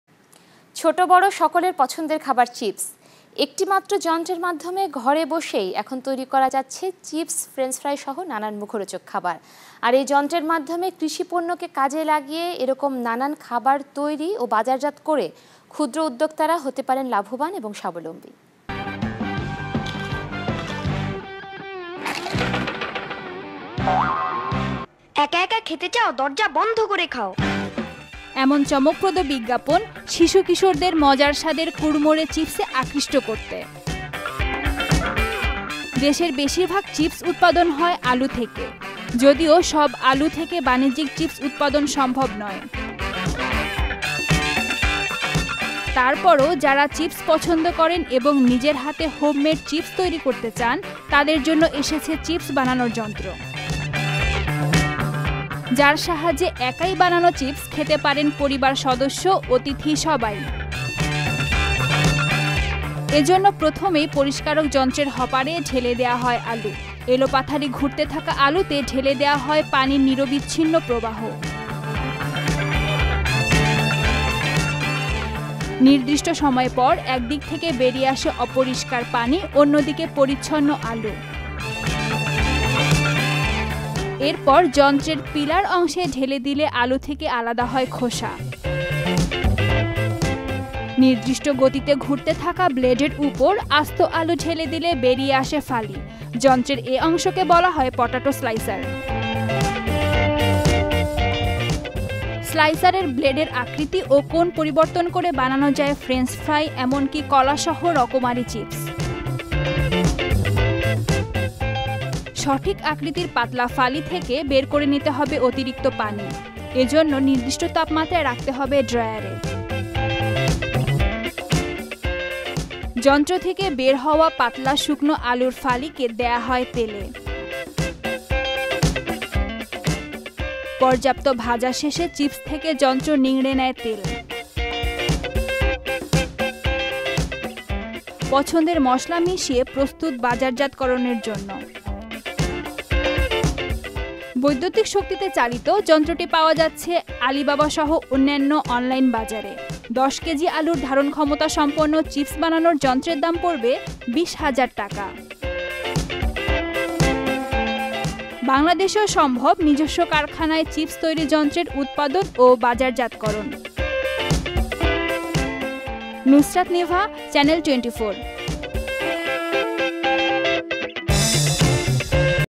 যন্ত্রের মাধ্যমে ঘরে বসেই এখন তৈরি করা যাচ্ছে চিপস
ছোট বড় সকলের পছন্দের খাবার চিপস। একটি মাত্র যন্ত্রের মাধ্যমে ঘরে বসেই এখন তৈরি করা যাচ্ছে চিপস, ফ্রেন্স ফ্রাই সহ নানান মুখরোচক খাবার। আর এই যন্ত্রের মাধ্যমে  কৃষিপণ্যকে কাজে লাগিয়ে নানান মুখরোচক খাবার তৈরি ও বাজারজাত করে ক্ষুদ্র উদ্যোক্তারা হতে পারেন লাভবান ও স্বাবলম্বী ।